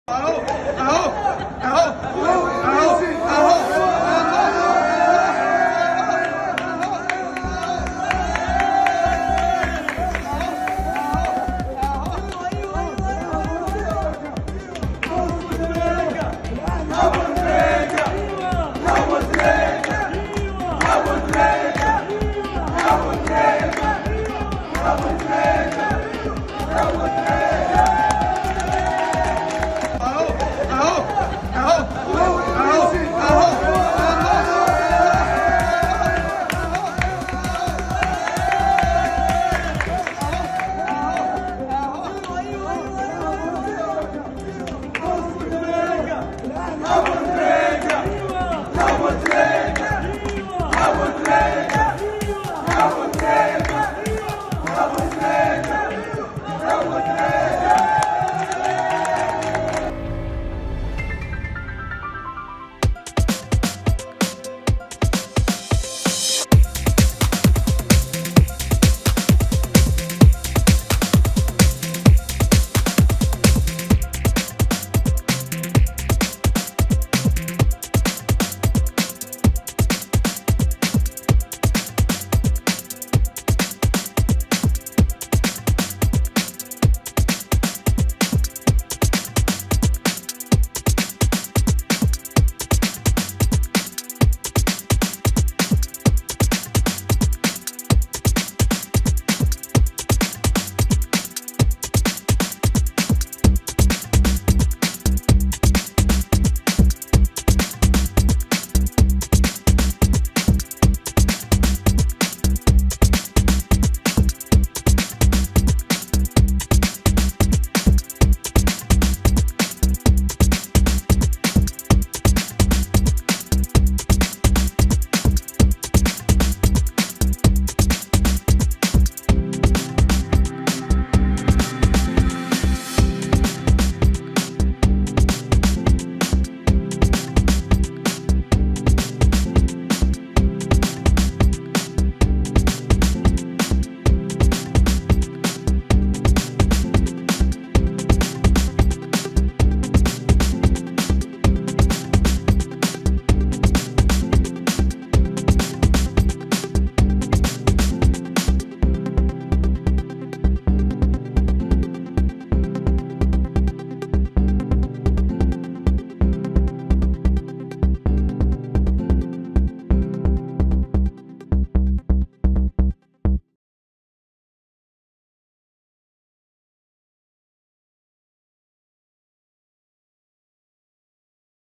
أبو تريكة.. أبو تريكة".. مهارات مشجع مصري تبهر الجماهير السعودية في الدوحة"
خطف مشجع مصري الأضواء خلال تواجده في أحد مطارات دولة قطر، وسط بعض الجماهير السعودية التي هتفت لمحمد أبوتريكة لاعب منتخب مصر والأهلي السابق.

أبوتريكة#
منتخب_السعودية#
السعودية_والأرجنتين#
مونديال_قطر#
Road_To_Qatar#
كأس_العالم_2022#